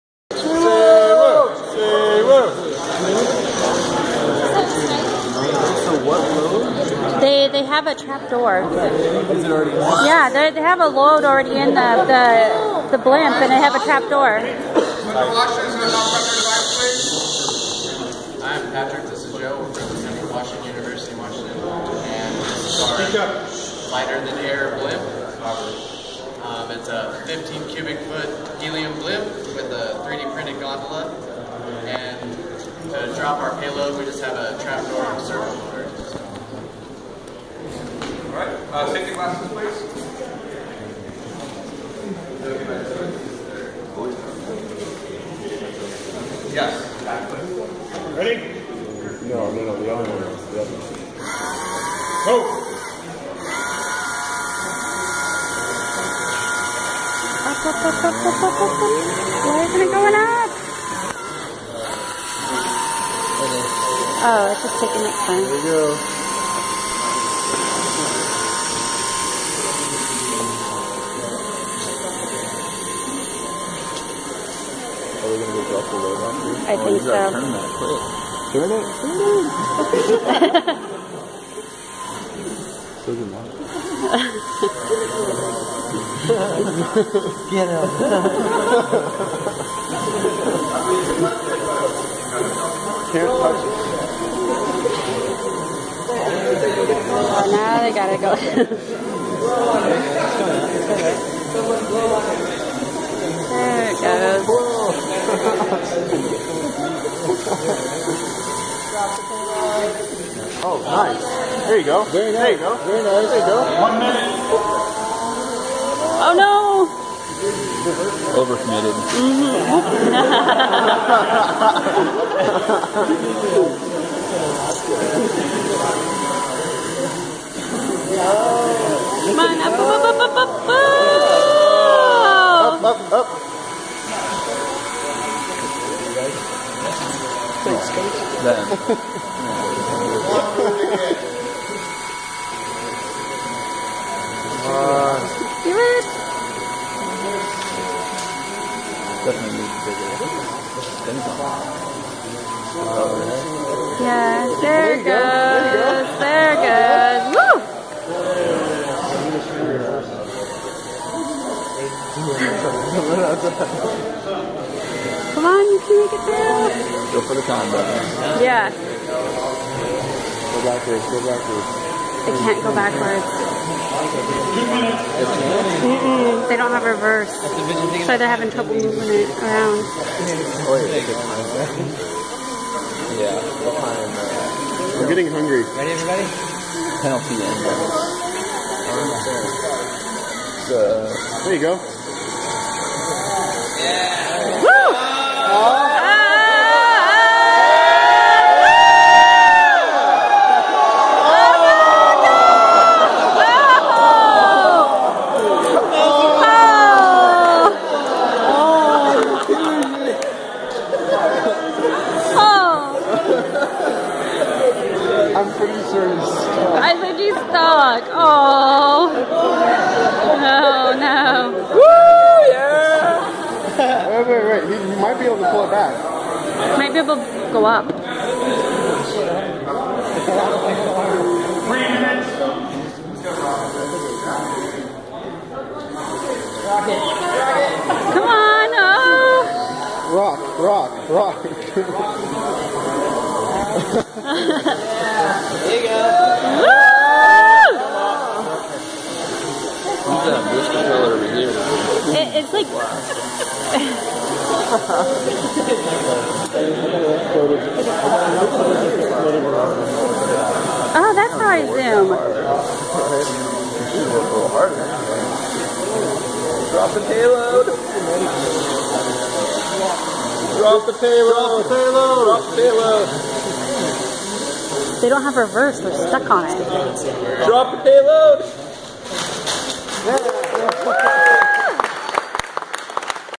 Lighter-Than-Air Blimp UAV
Footage of our UAV from the ASME Student Design Challenge at Cal Poly. The balloon lost helium when it was in quarantine and did not perform as expected during the competition.